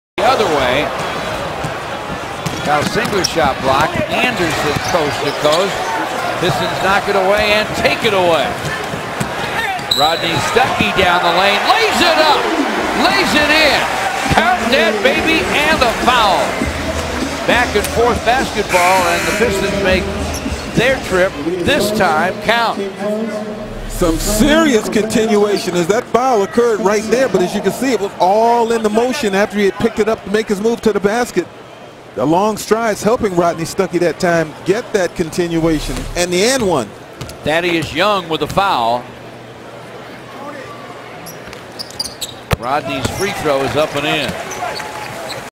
Rodney Stuckey And1 Left Hand Layup
Detroit Pistons  Vs Philadelphia 76ers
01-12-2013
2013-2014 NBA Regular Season